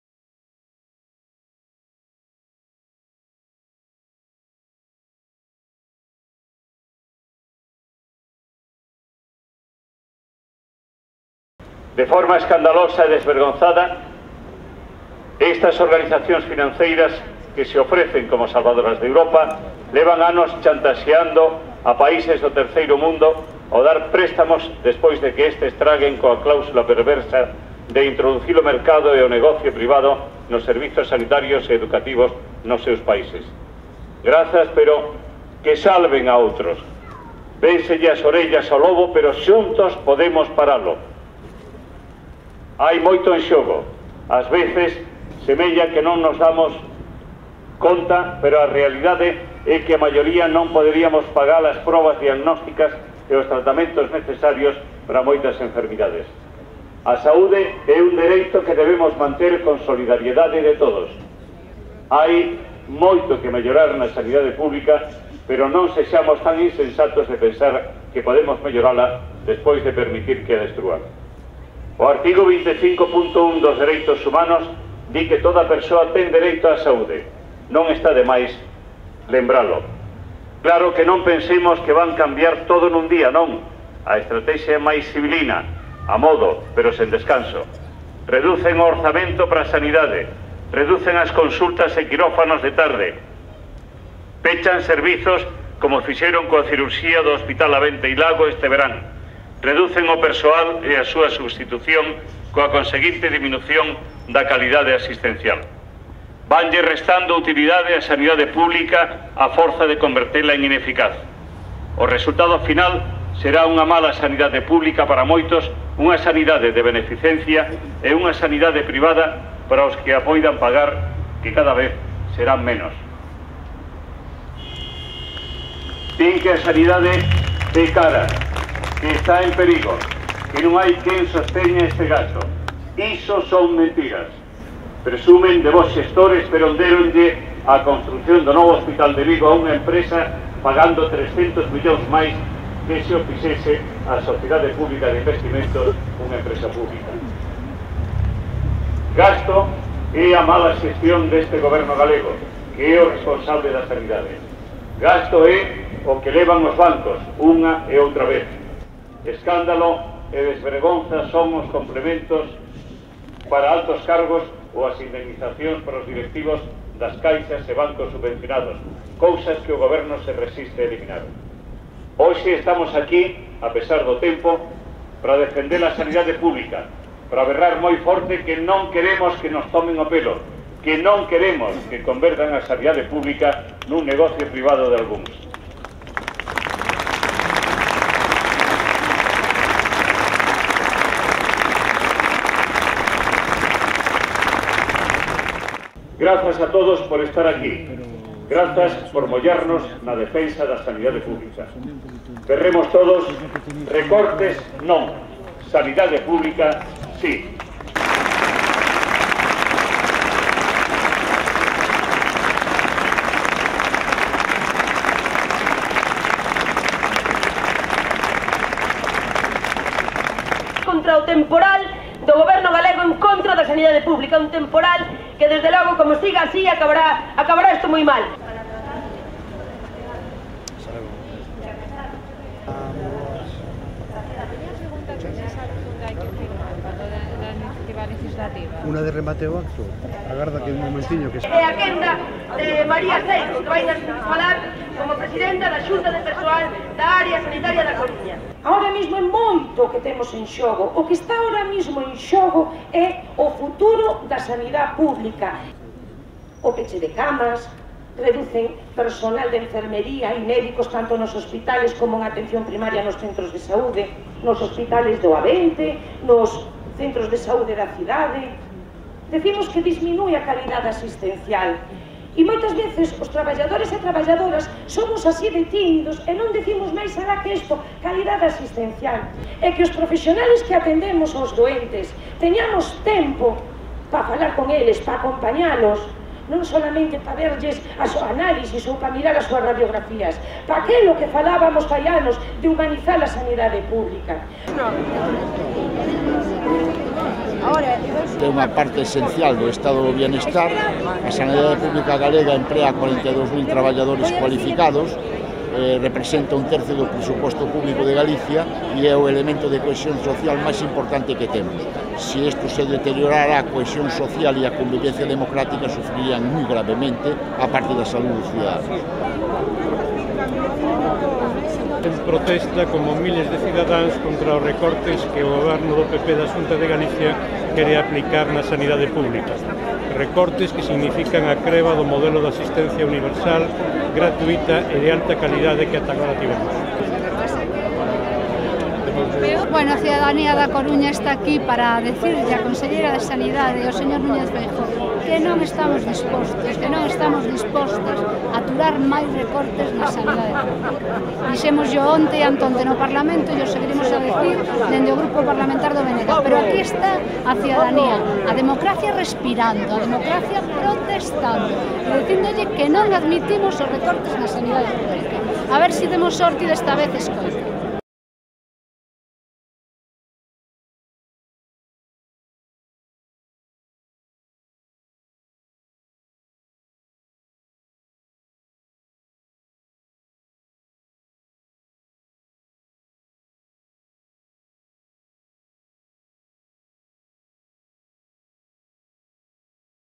S.O.S Sanidade Publica
Concentración na Coruña o xoves 27 de outubro do 2012